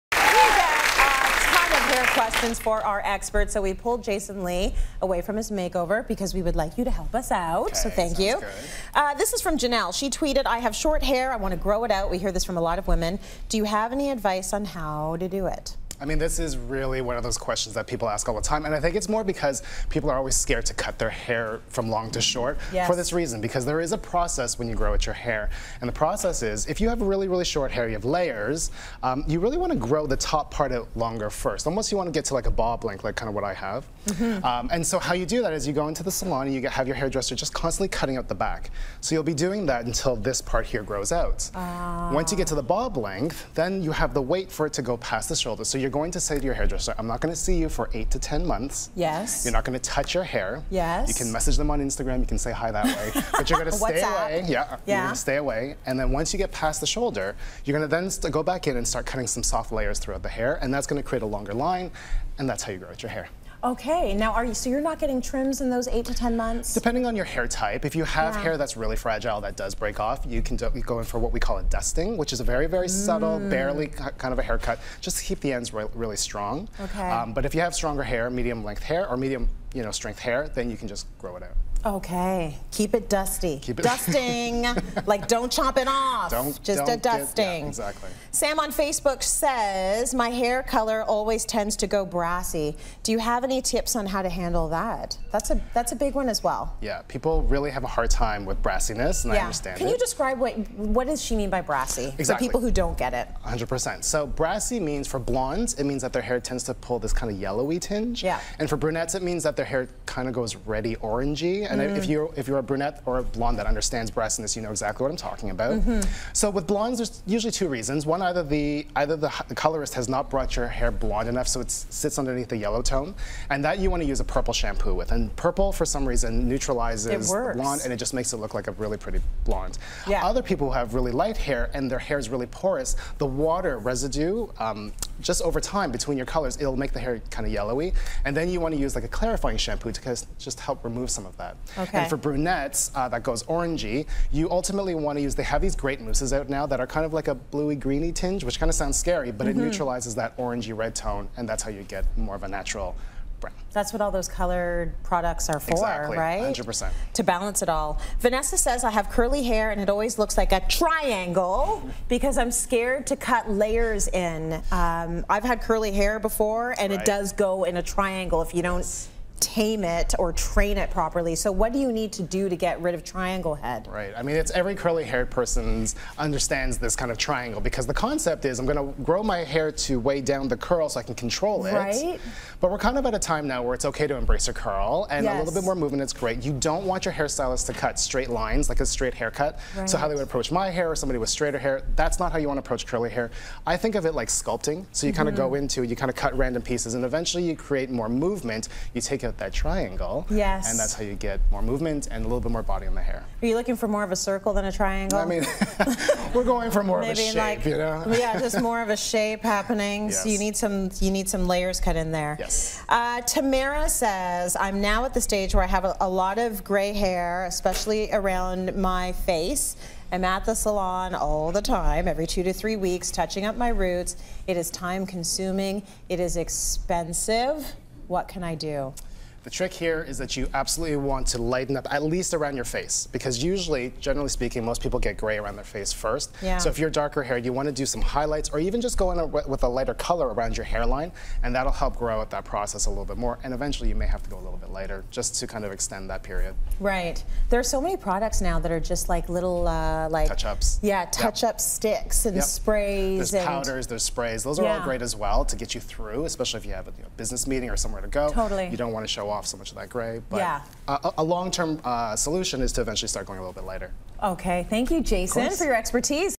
Watch this before you try to grow your hair long
Before you go to the salon you need to hear this! Hair Stylist Jason Lee answers your top hair styling questions.